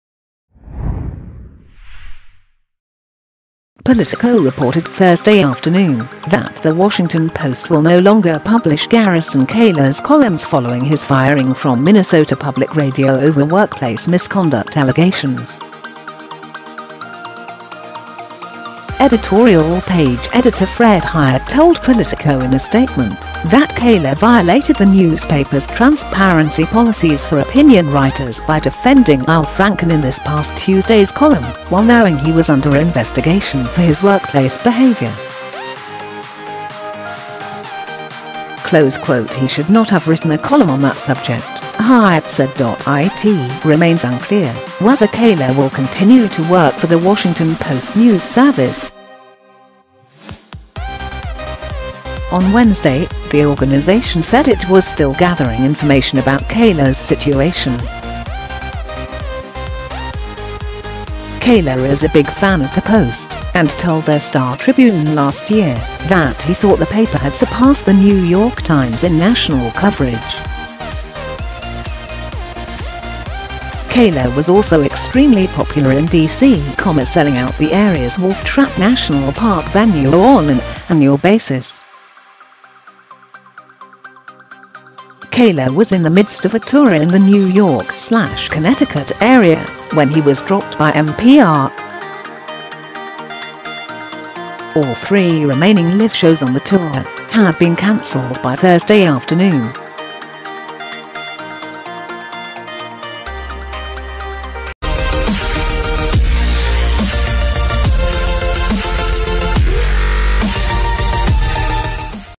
Garrison Keillor dropped by Washington Post; all tour shows canceled
Daily politic today. Garrison Keillor dropped by Washington Post; all tour shows canceled
Politico reported Thursday afternoon that The Washington Post will no longer publish Garrison Keillor’s columns following his firing from Minnesota Public Radio over workplace misconduct allegations.Editorial page editor Fred Hiatt told Politico in a statement that Keillor violated the newspaper’s transparency policies for opinion writers by defending Al Franken in this past Tuesday's column while knowing he was “under investigation for his workplace behavior.”He “should not have written a column on that subject,” Hiatt said.It remains unclear whether Keillor will continue to work for the Washington Post News Service & Syndicate, which distributes columns to newspapers nationwide.On Wednesday, the organization said it was still gathering information about Keillor's situation.Keillor is a big fan of the Post and told the Star Tribune last year that he thought the paper had surpassed the New York Times in national coverage.Keillor was also extremely popular in DC.,selling out the area's Wolf Trap National Park venue on an annual basis.Keillor was in the midst of a tour in the New York/Connecticut area when he was dropped by MPR.All three remaining live shows on the tour had been canceled by Thursday afternoon.
Source Photo and Content:DailyMail, REUTERS, AP, EPA, Getty Images etc.
Daily politic today channel dedicated to sharing the latest news around the world.